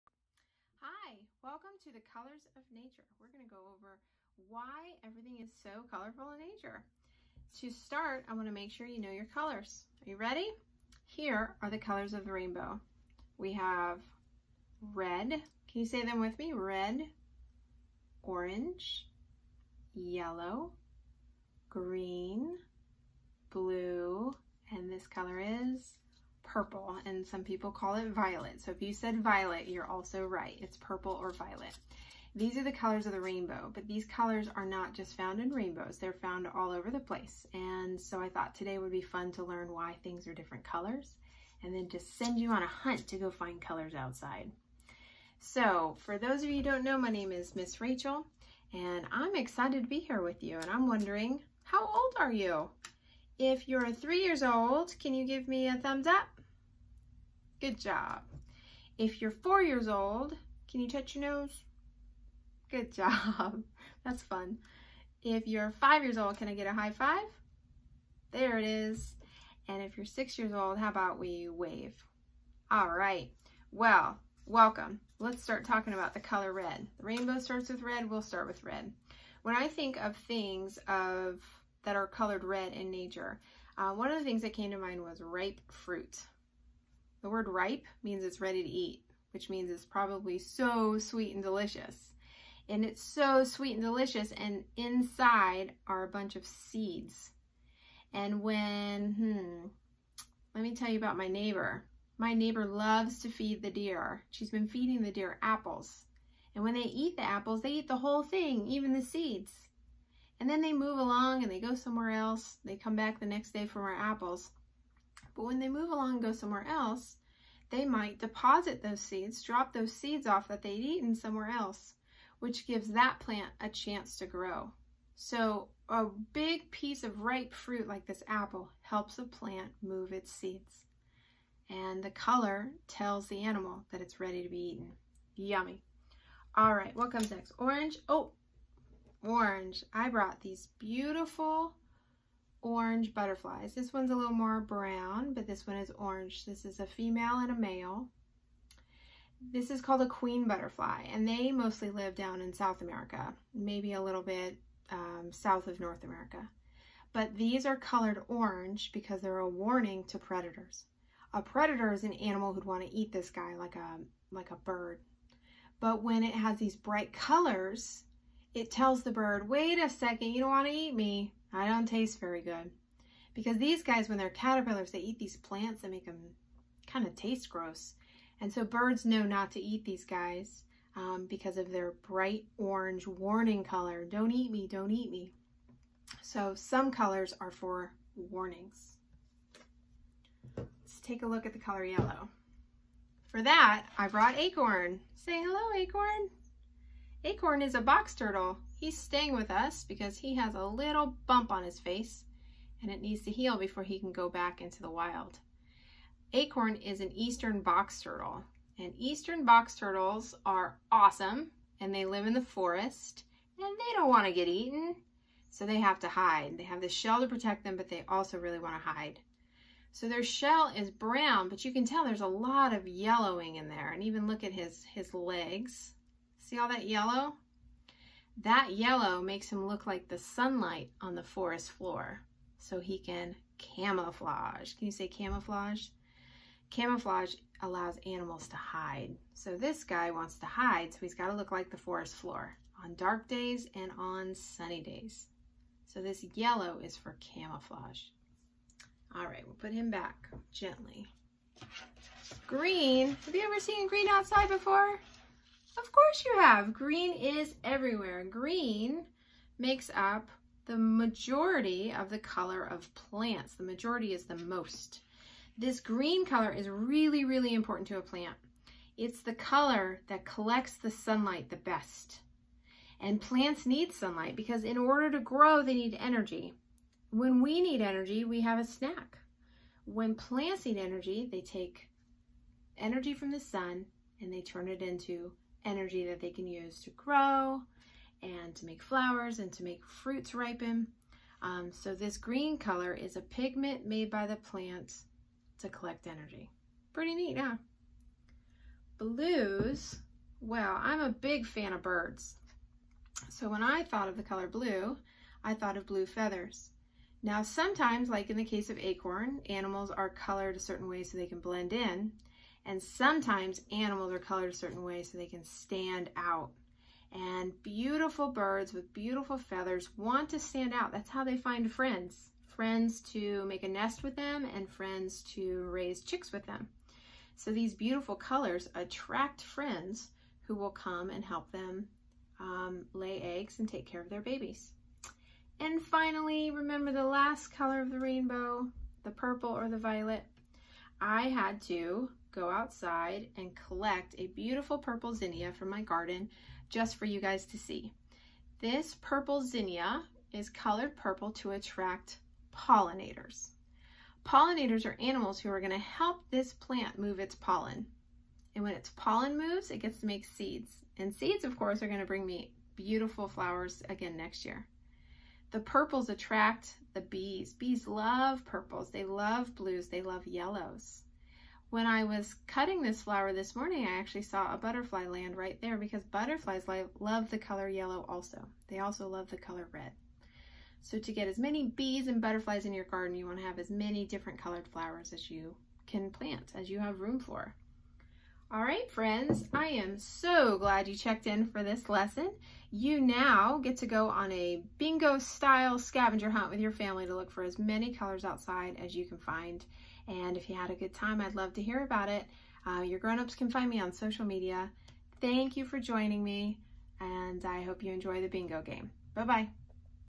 The Colors of Nature
An introductory video to accompany the color scavenger hunt outdoor adventure which will sharpen a child's observation skills, interest in nature and understanding of living things. Designed for 3-6 year olds.